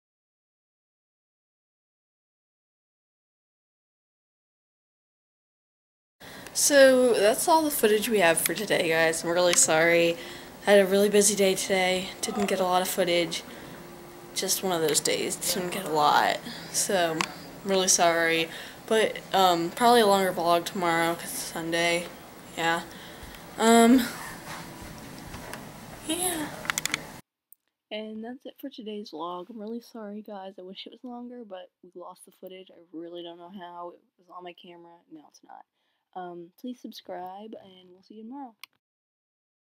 WHERE'S THE FOOTAGE?! (Rap squad family vlogs DAY 14)
Rap squad family vlogs DAY 14! People, we lost a lot of footage today.